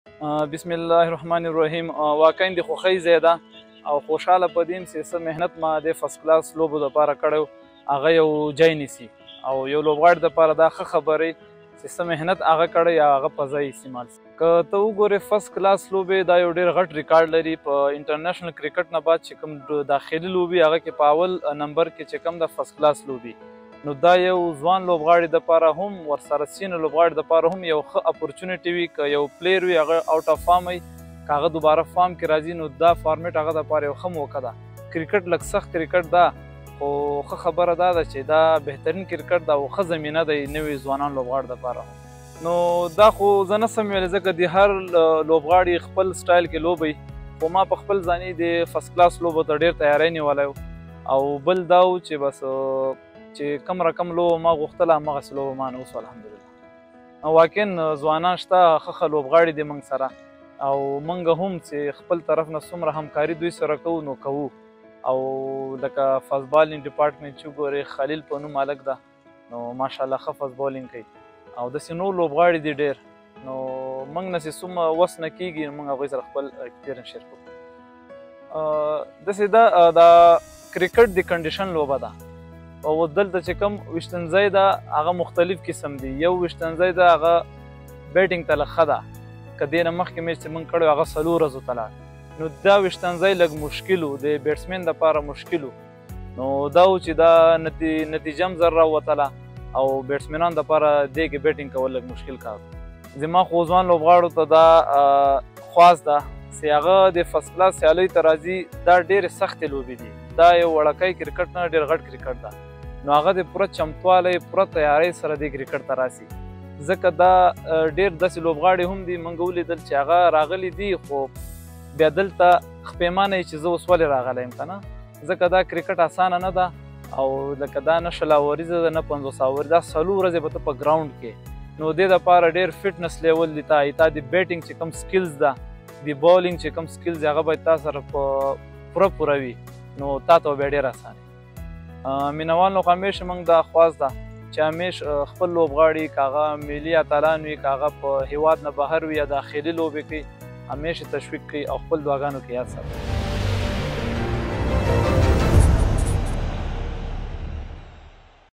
Karim Janat reacts on his all-round performance against Amo Region | AbdaliFCT2022 | ACB
Karim Janat, once again, put on a brilliant all-round performance as he scored a 116 runs in the first inning and took 3 wickets in the 2nd inning and was subsequently awarded player of the match.